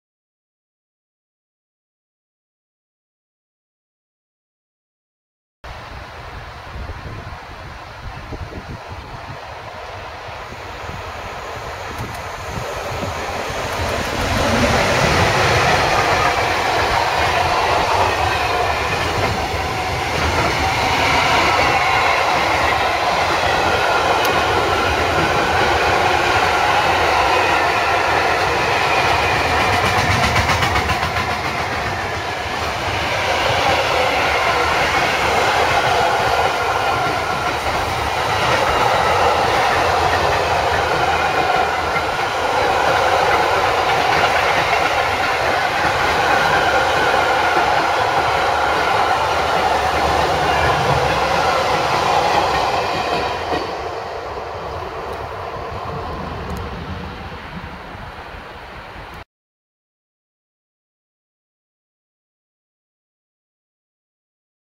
DB 185 324-8 med godstog
DB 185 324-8 med godstog passerer Kolding den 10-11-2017